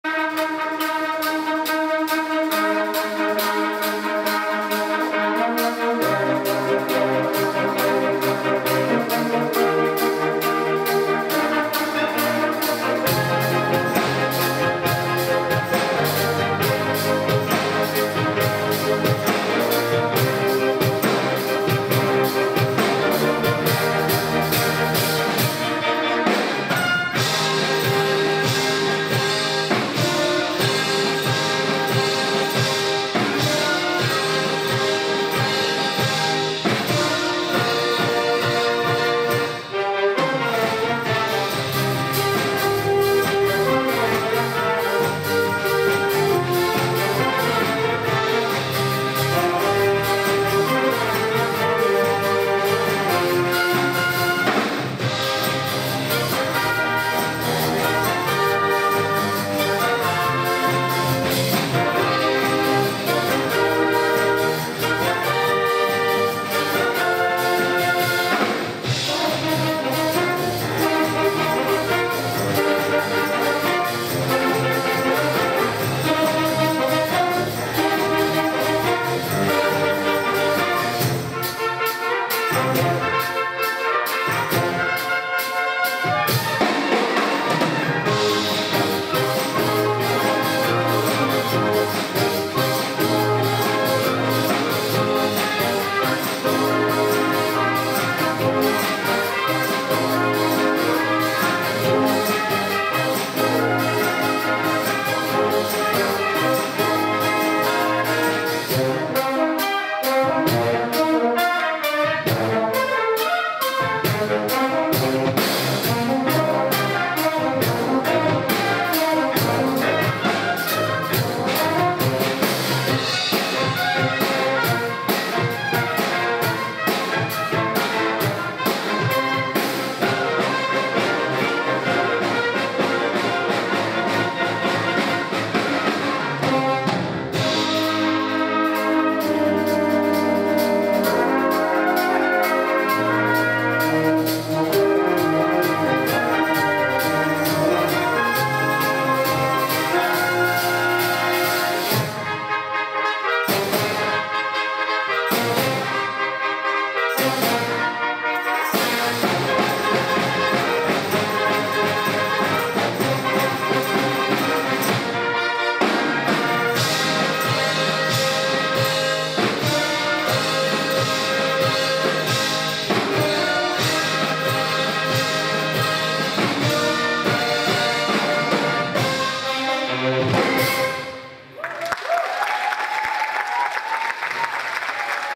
11.07. XXIV Balkan folk fest Orkiestra Reprezentacyjna AGH 24
11.07. XXIV Balkan folk fest
XXIV Balkan Folk Fest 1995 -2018 Bulgaria, Varna, Black sea
Фестивален и конгресен център - Варна